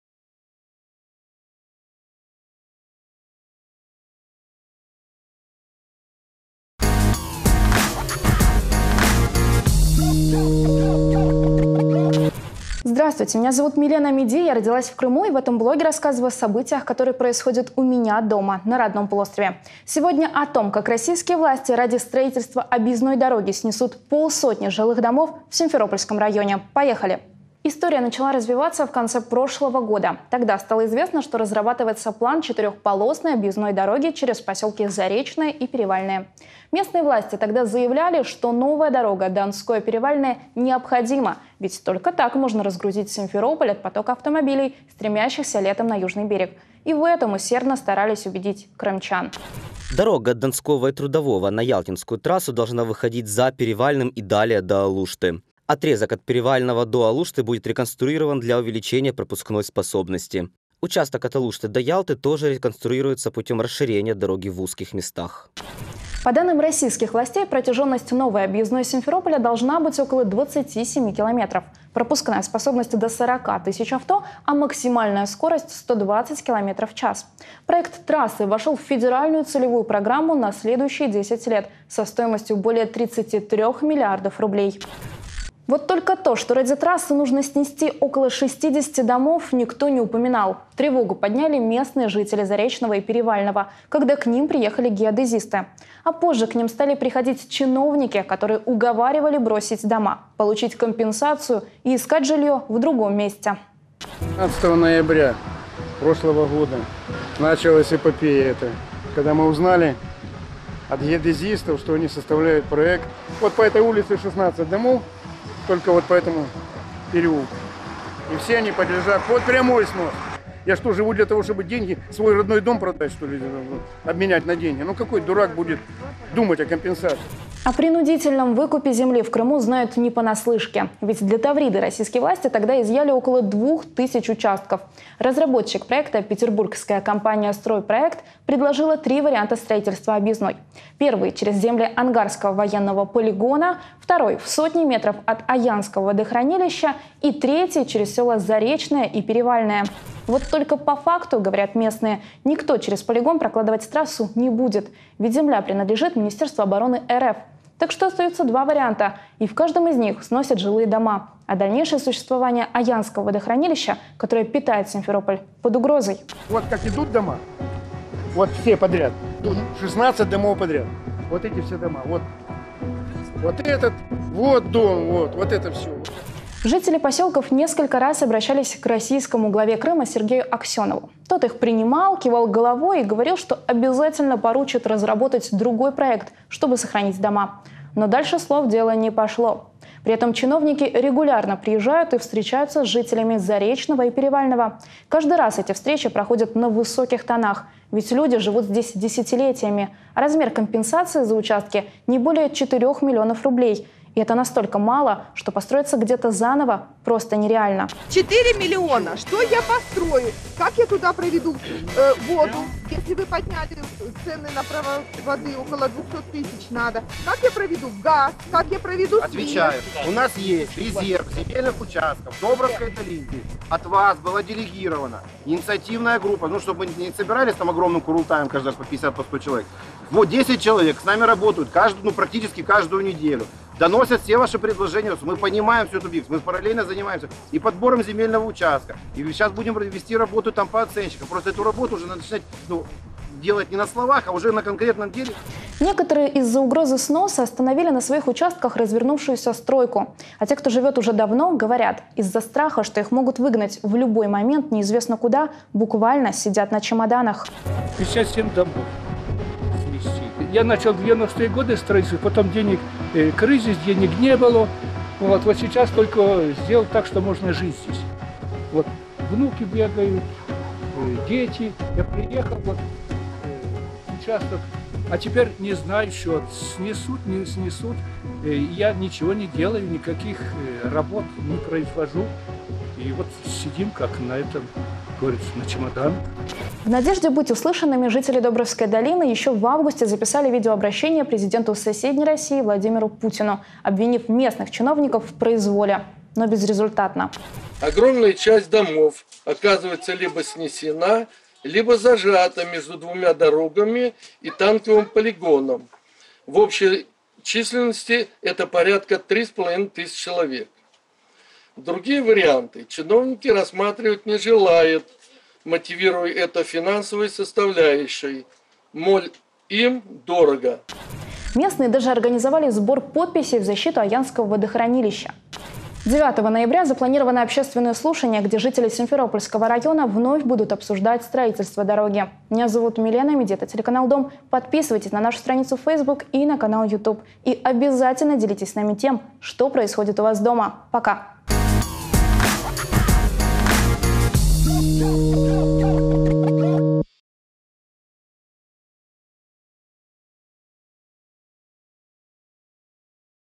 "Власти" Крыма снесут жильё ради дороги: под угрозой 50 домов
Ради строительства объездной дороги российские власти Крыма собираются снести полсотни жилых домов в Симферопольском районе. Об этом проекте стало известно в конце прошлого года. Крымчан убеждали, что объездная дорога поможет разгрузить Симферополь от потока авто. Вот только о том, что для её строительства нужно будет снести более полусотни домов, никто не предупреждал.
Все подробности этой истории смотрите в блоге журналистки из Крыма Милены Амеди.